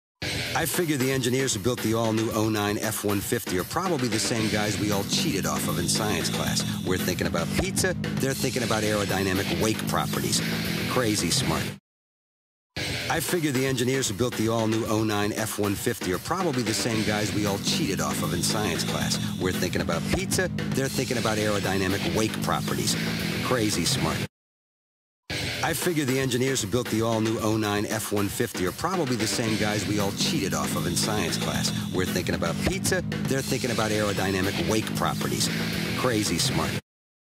Ford F-150 Commercial REMAKE (Comparison)
I remade the text in this Ford commercial using Adobe After Effects. Crazy Smart.